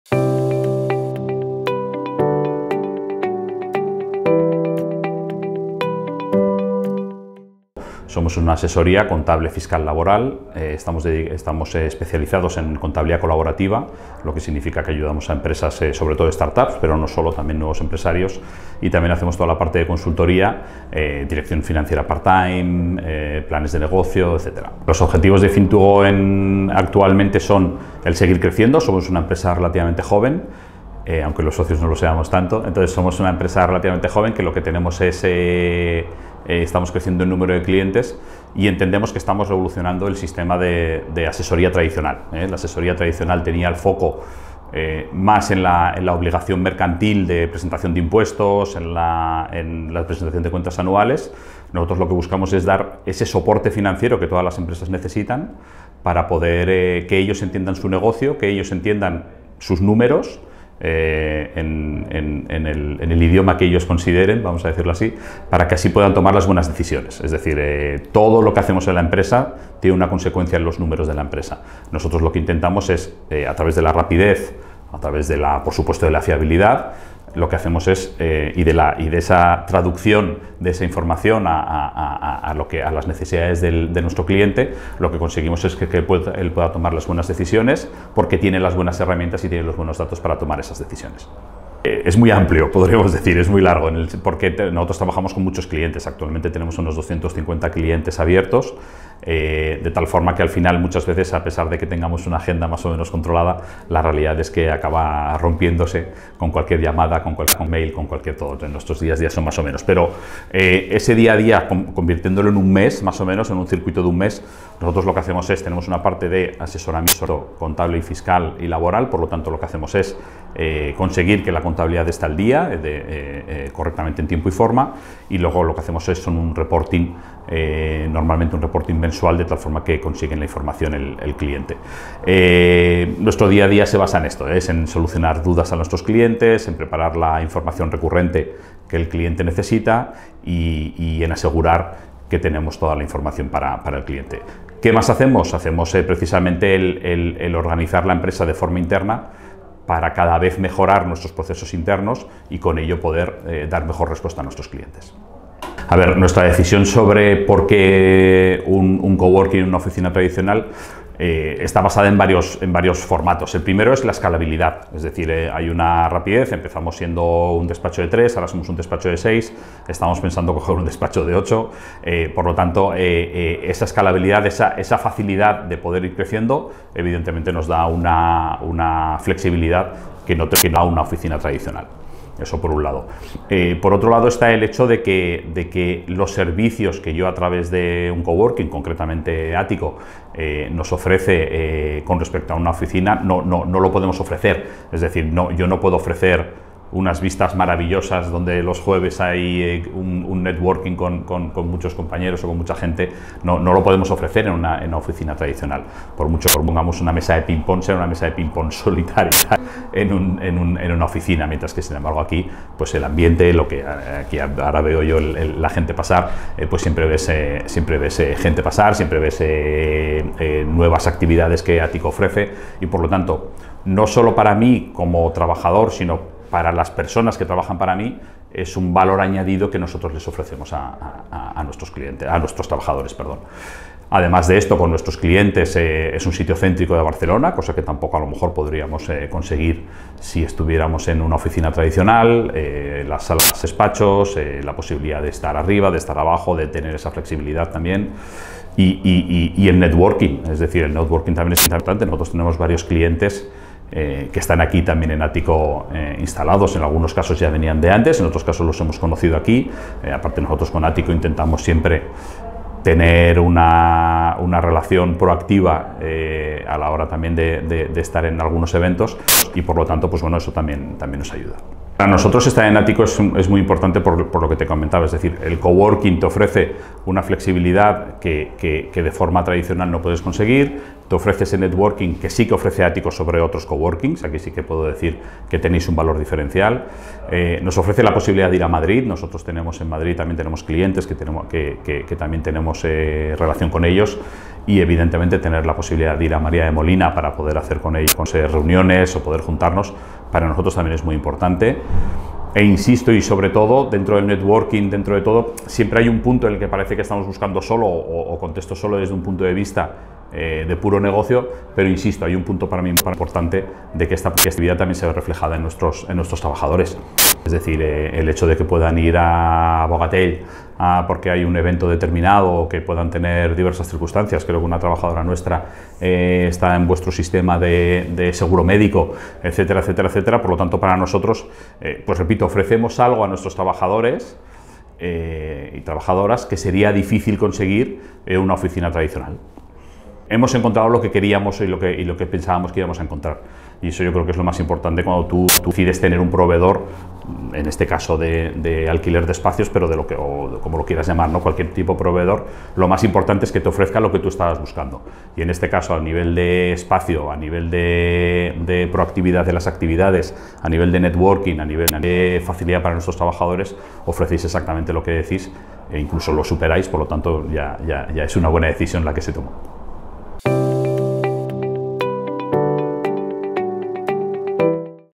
Meet our Community | Fin2gO
Una de las cosas caracterizan a Aticco, es su comunidad, donde convergen profesionales, emprendedores y empresas top que nos motivan a hacer más y mejor para ayudarles a crecer y alcanzar sus objetivos. Una comunidad que nos enorgullece y que queremos que conozcas. En esta edición de Meet our Community queremos contarte sobre la asesoría Fin2gO.